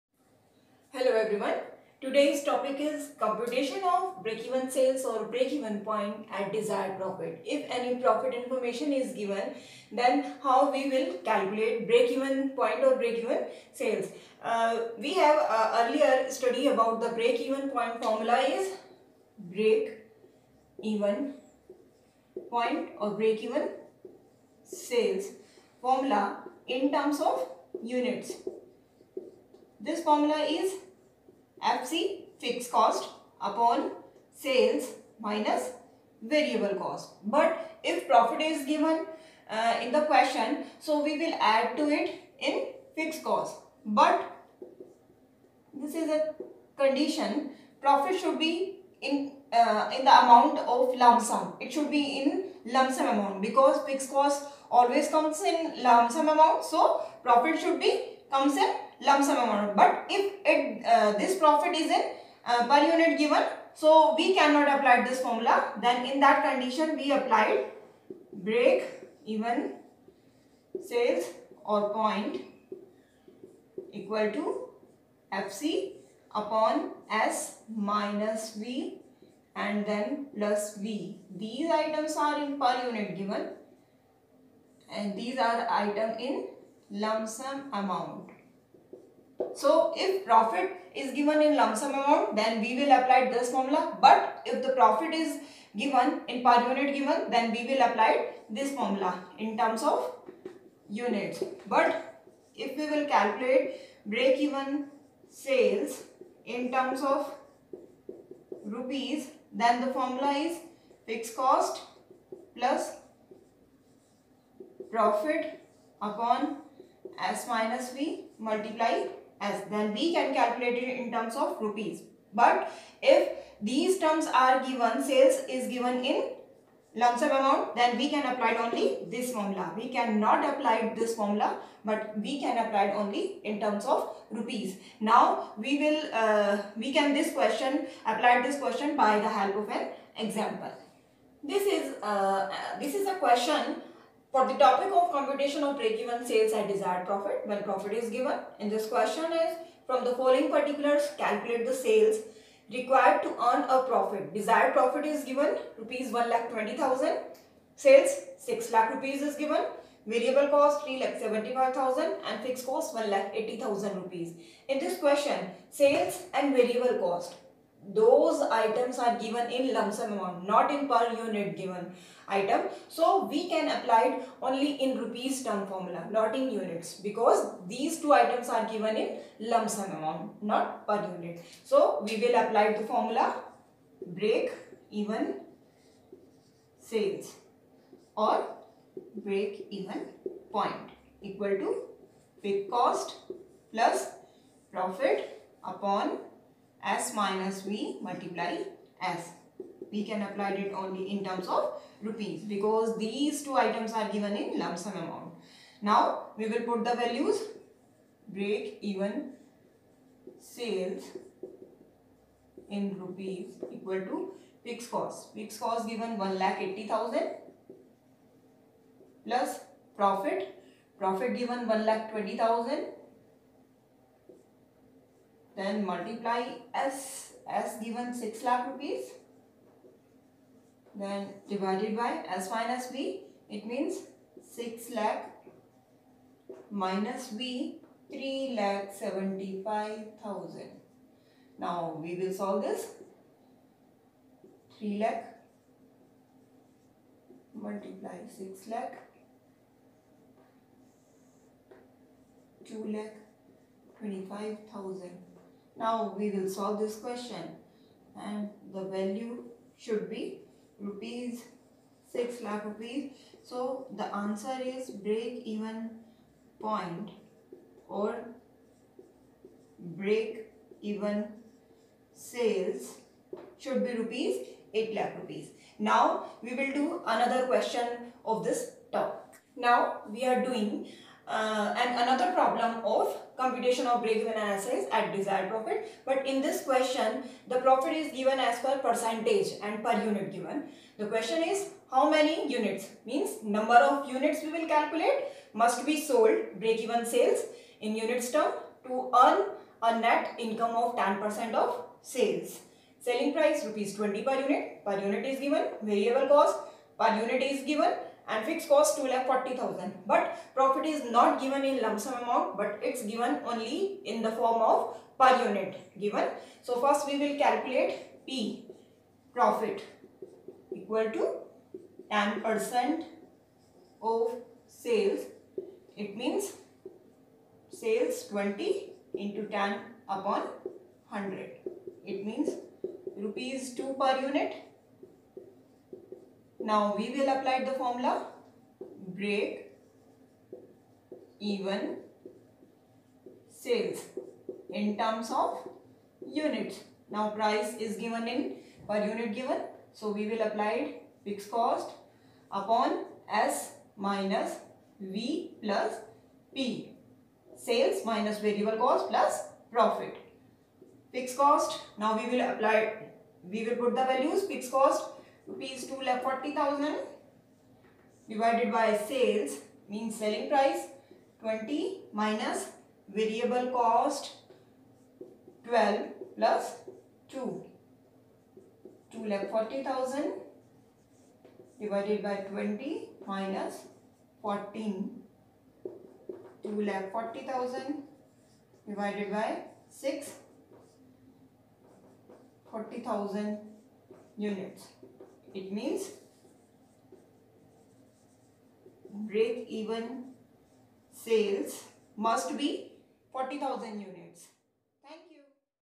Computation of Break Even Sales at Desired profit.
Useful for B.com.,M.com., BBA, MBA Students.
Computation of Break even analysis/Break even sales, when desired profit is given in per unit /lumsum.